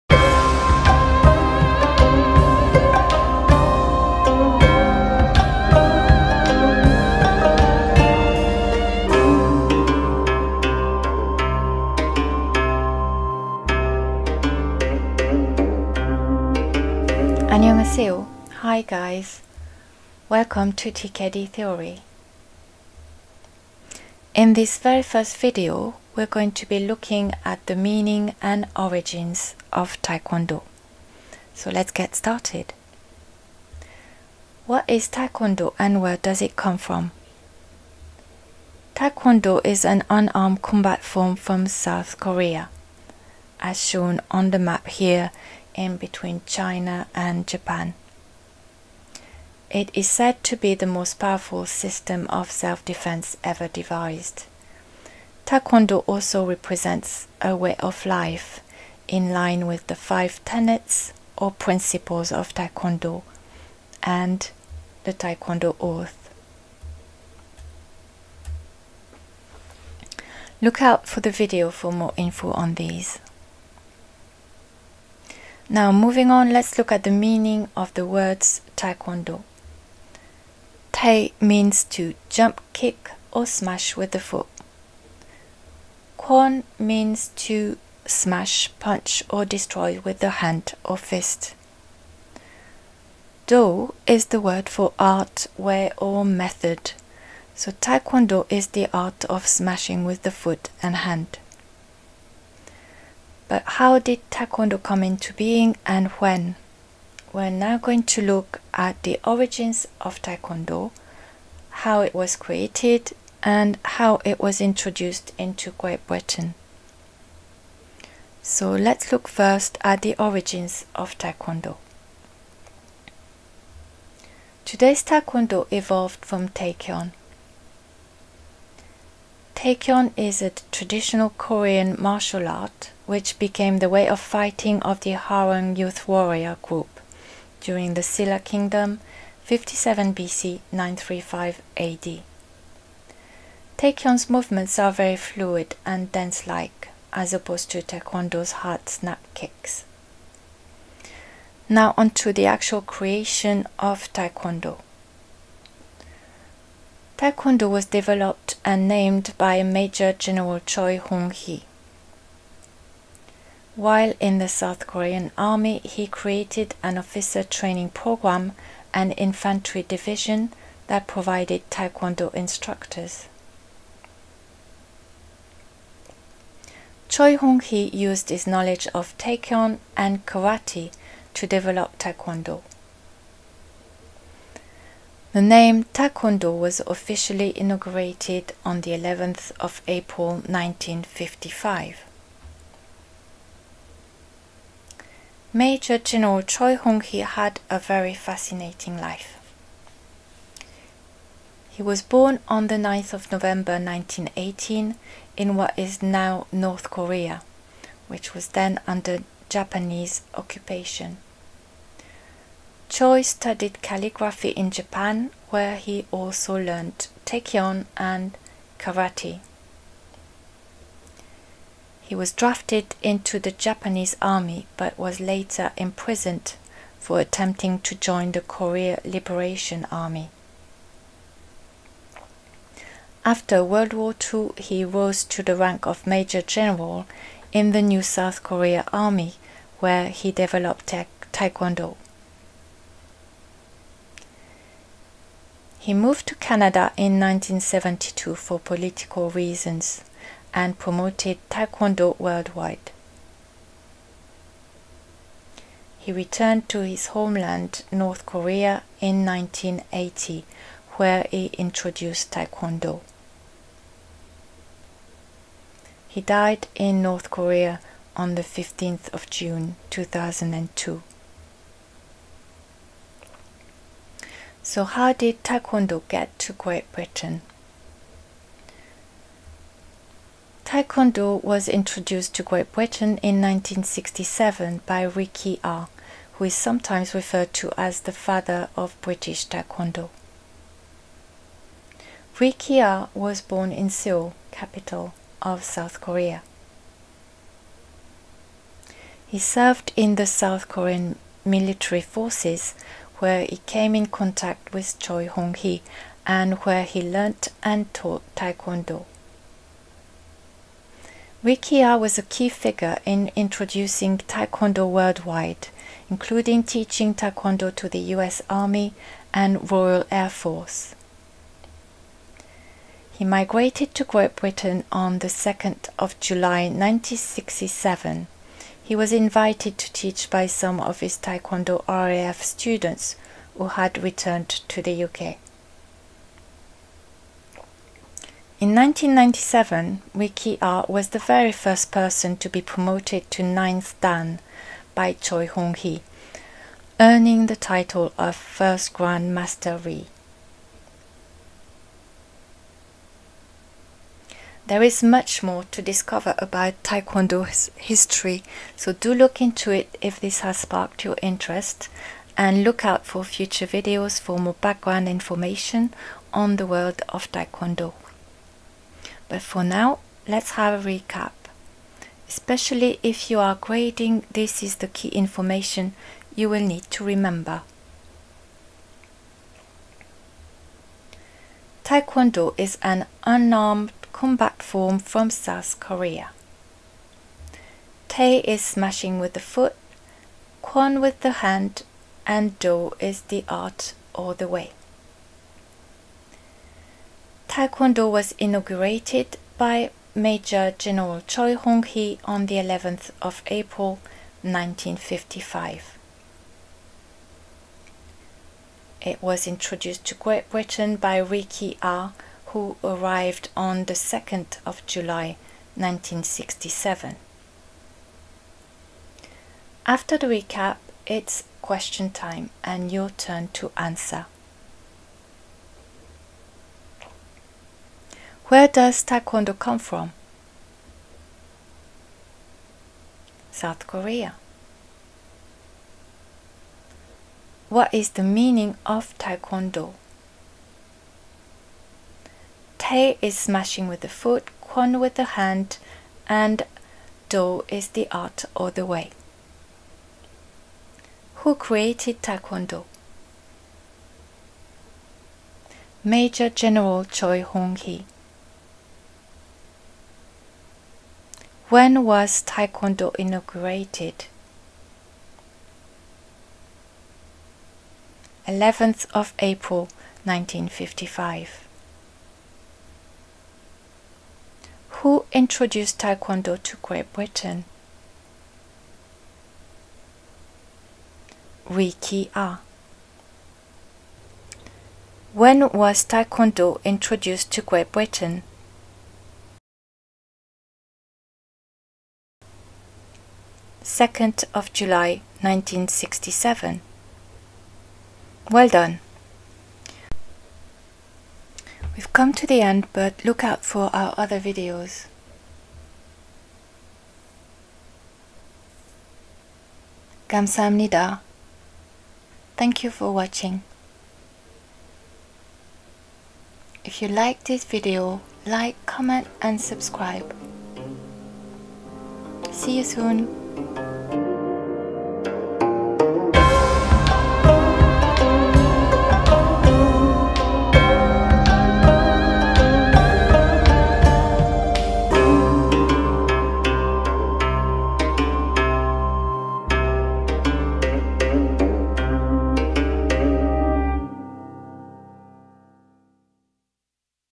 Meaning and origins of Tae Kwon-Do
Tae Kwon-Do is an unarmed combat form from South Korea. It is said to be the most powerful system of self-defence ever devised. Find out more about its origins.

Image credits:
- Hyesan Yu Suk (유숙)(1827–1873), Public domain, via Wikimedia Commons